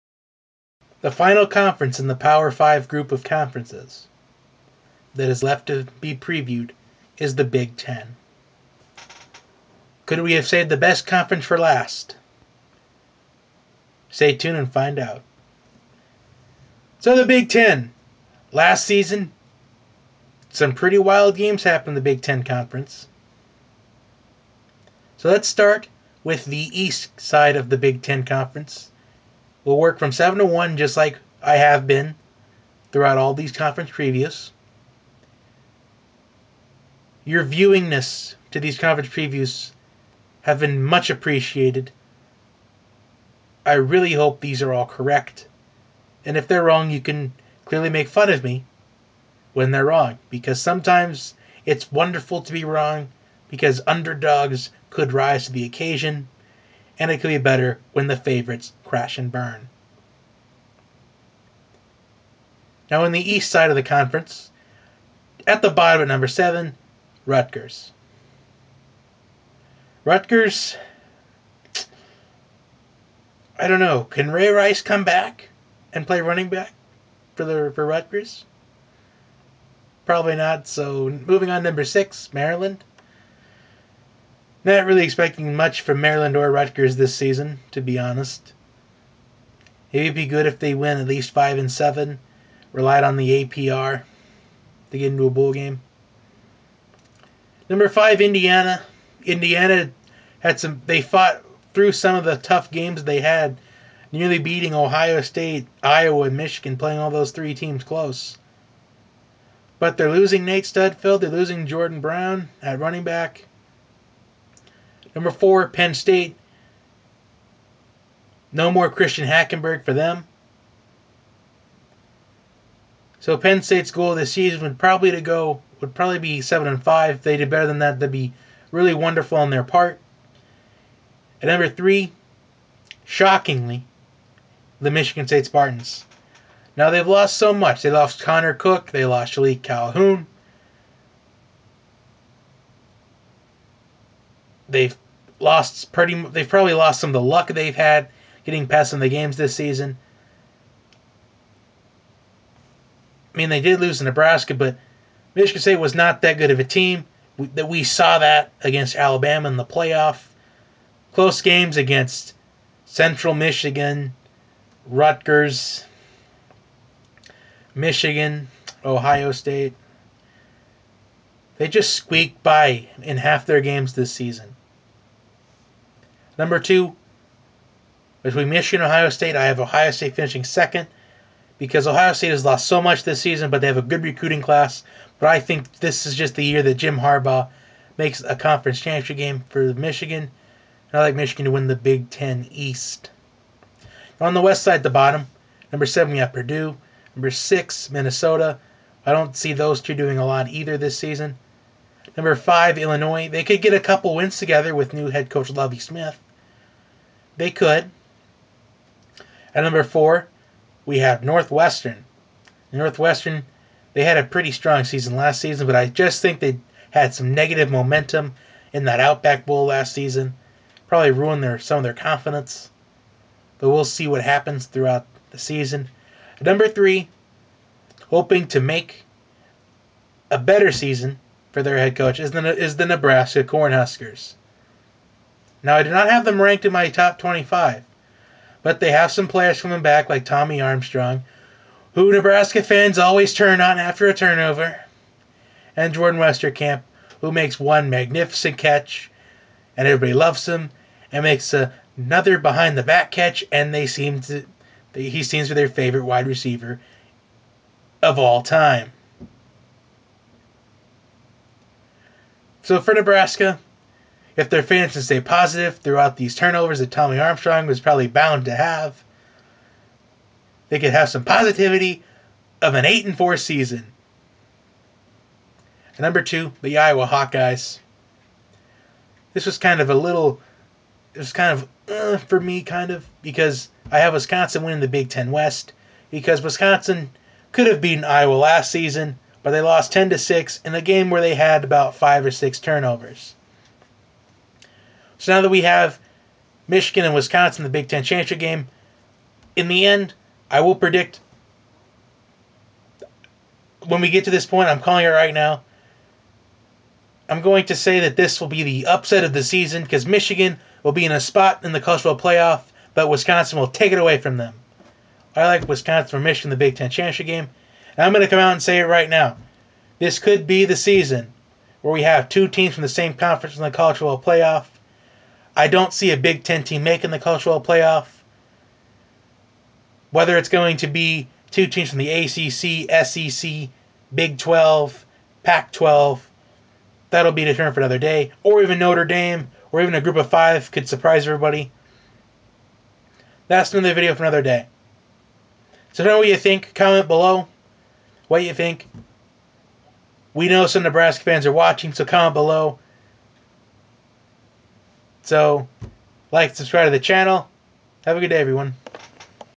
2016 Big 10 Conference Preview
We end our last power 5 conference previews with the Big 10